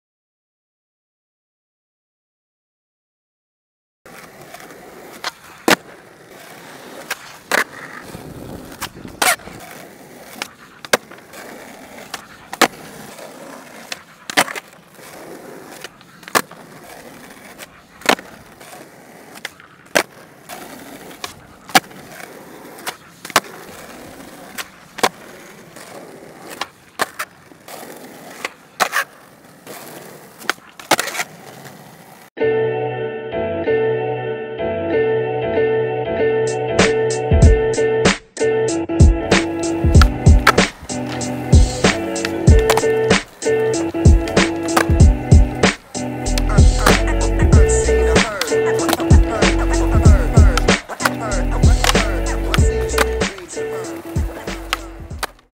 FLAT GROUND TRICKS #17 - MIKE PIWOWAR
Flat ground tricks starring Mike Piwowar.

Tricks: Impossible, Bs Bigspin, Switch Bs Bigspin, Varial Heel Flip, Switch Varial Heel Flip, Varial Heel Flip Fakie, Inward Heel Flip, Switch Inward Heel Flip, Double Flip, Switch Double Flip, 360 Flip, Switch 360 Flip, 360 Ollie, Switch 360 Ollie, Fakie 360 Heel Flip.